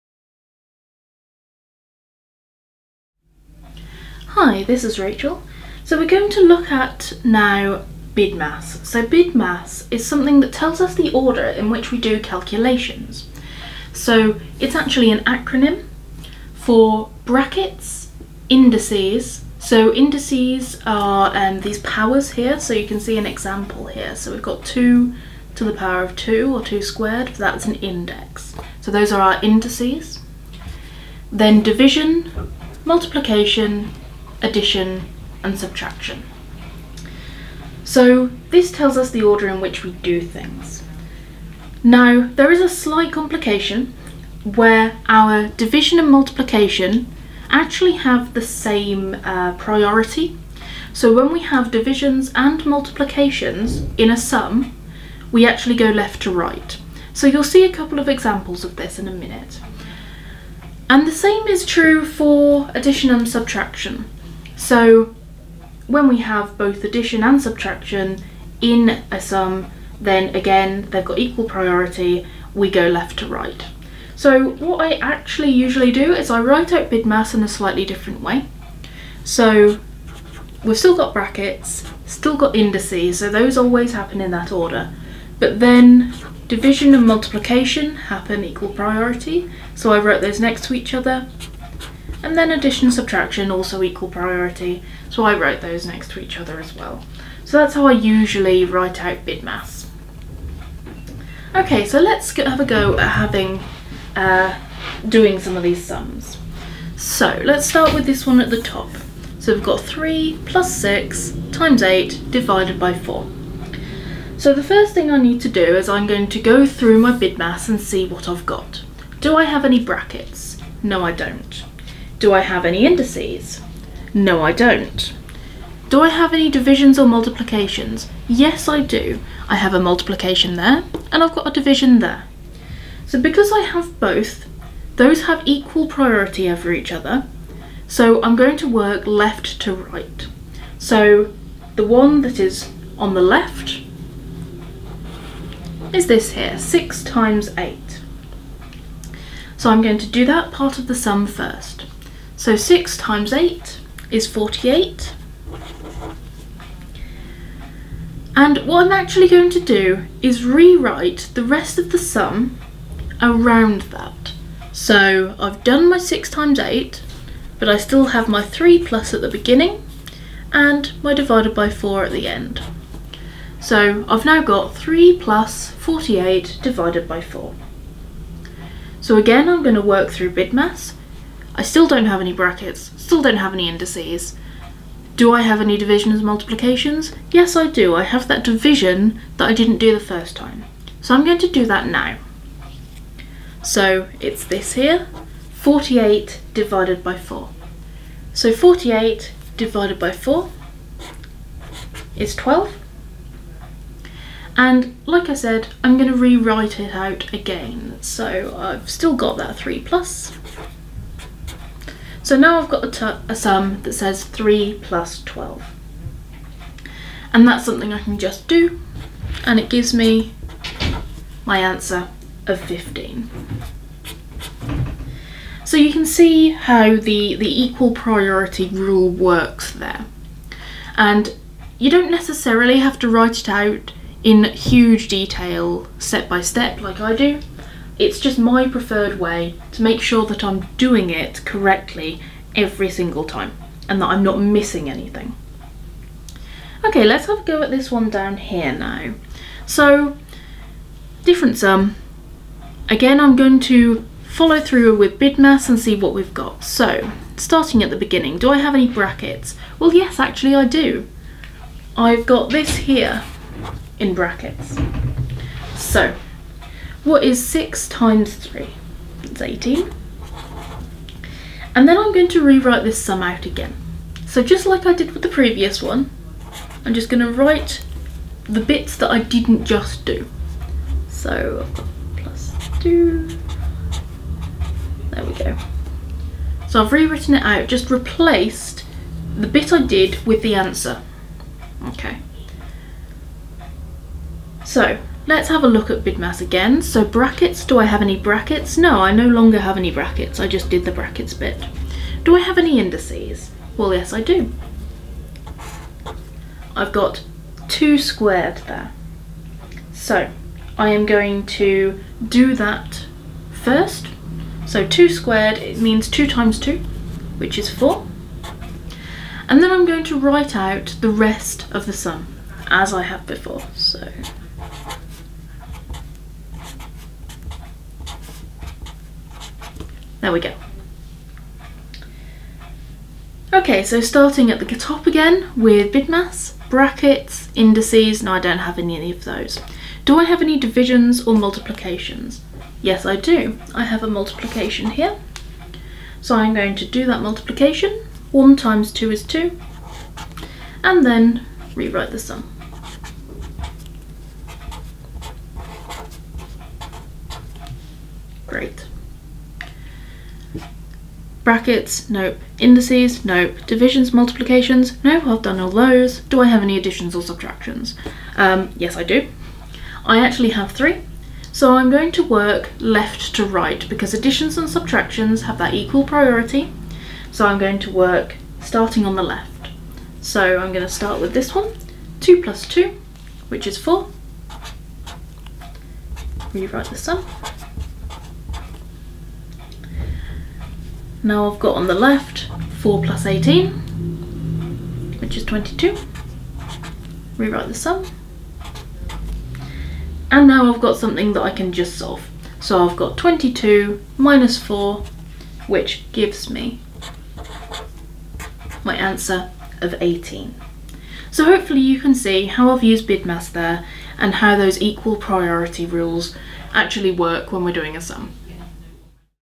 BIDMAS
Rachel explains how to apply BIDMAS (the order of operations) to calculations.

BIDMAS is also known as BODMAS or PEMDAS.